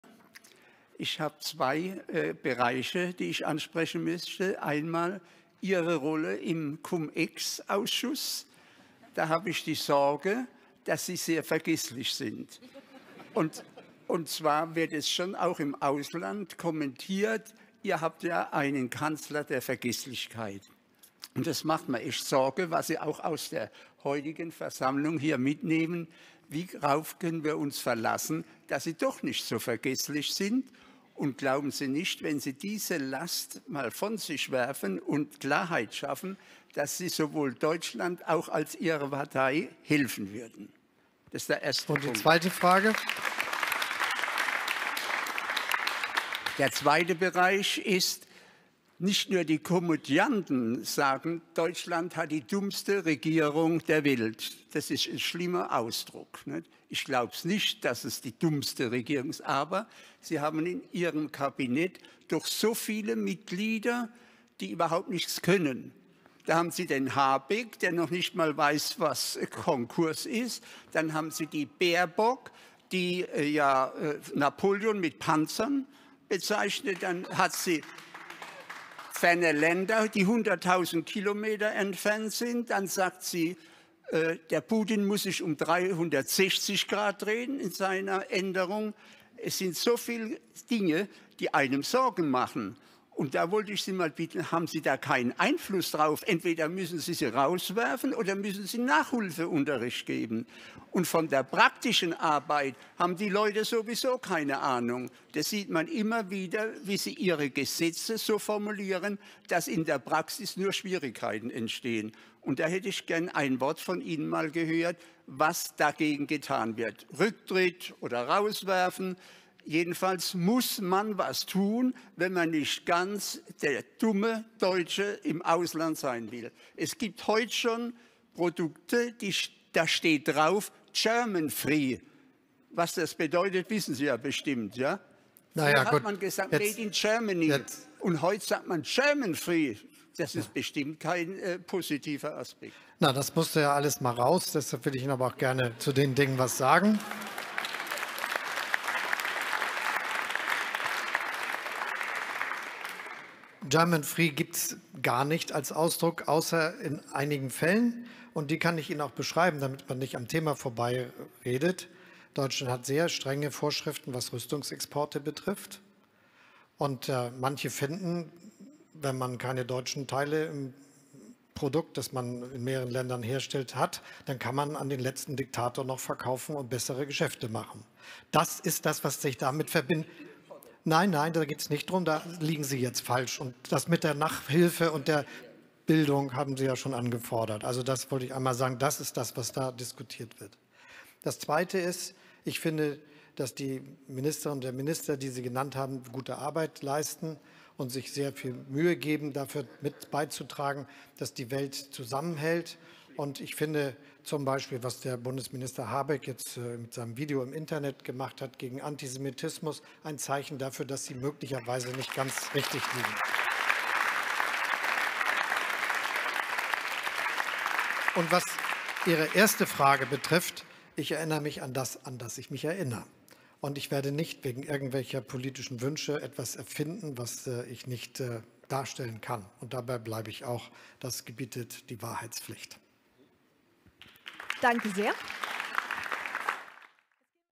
Opa stellt Scholz im Bürgerfoum zur Rede
Talk-Gast-Opa äußert sich zu Olaf's Gruselkabinett im Bürgerforum Mannheim. Der erklärt dem Opa, das Bärbock und Co sich aber so viel Mühe geben und dass man das mal anerkennen soll. Zu der vorgeworfenen Cum-Ex-Korruption kann er sich weiterhin nicht erinnern und beharrt auf seiner "Ehrlichkeit".